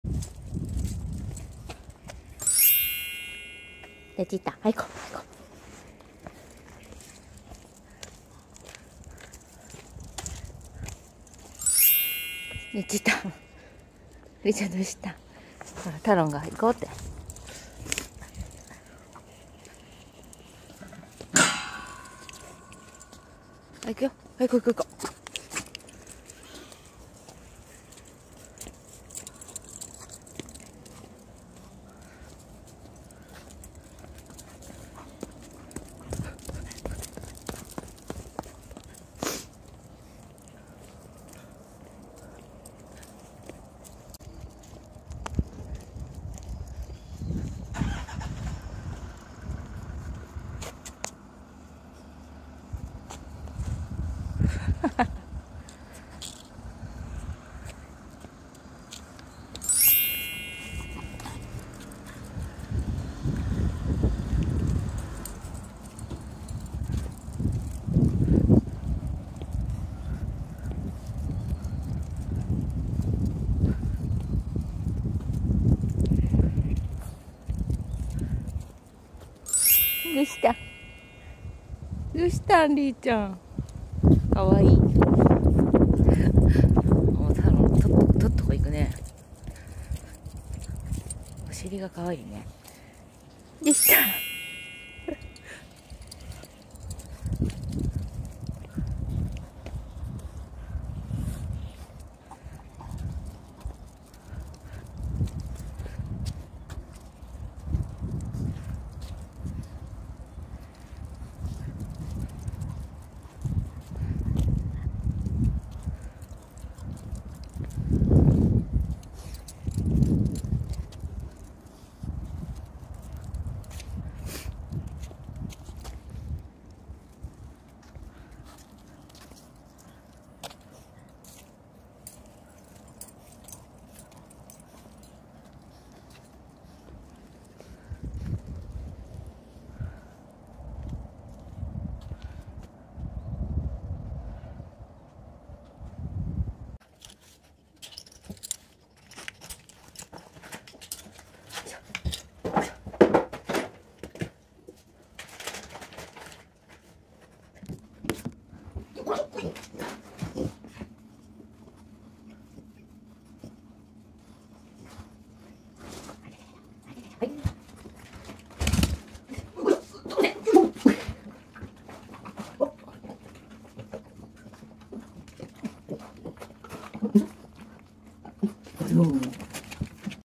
【あざとかわいい】フレンチブルドックタロンとリーお散歩
お散歩です。
2頭のお散歩はなかなか大変ですが
楽しくもあります。